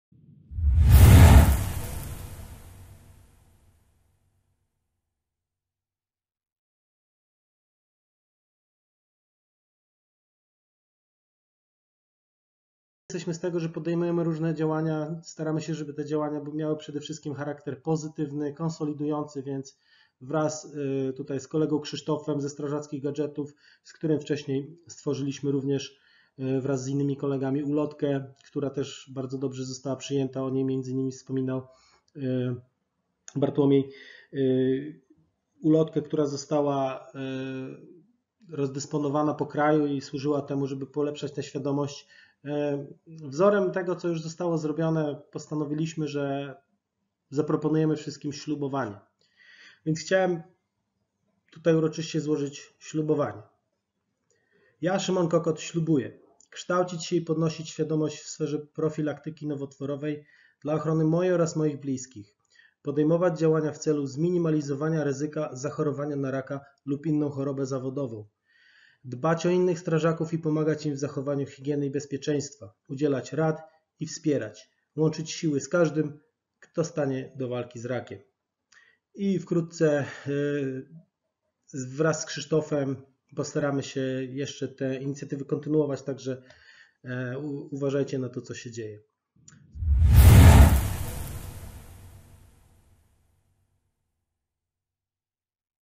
Ślubowanie - Nie dla raka w straży!
Fragment konferencji "Rak w straży pożarnej / Cancer in the fire services",
4 lutego 2021 - Miedzynarodowy Dzień Walki z Rakiem

Więcej informacji tu: